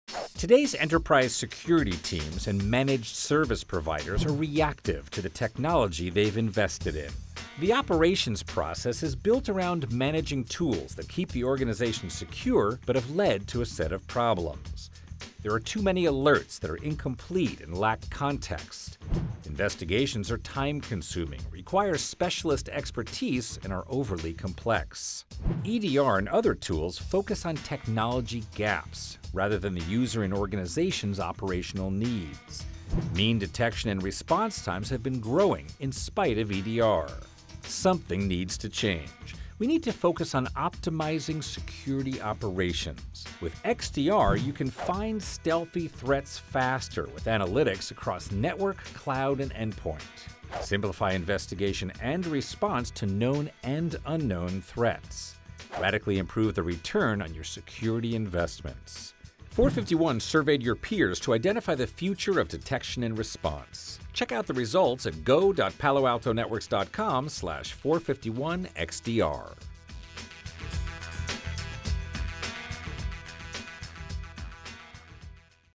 Optimizing Detection and Response for the SOC
Today’s operations process is reactive and broken. A shift must occur to focus on optimizing security operations.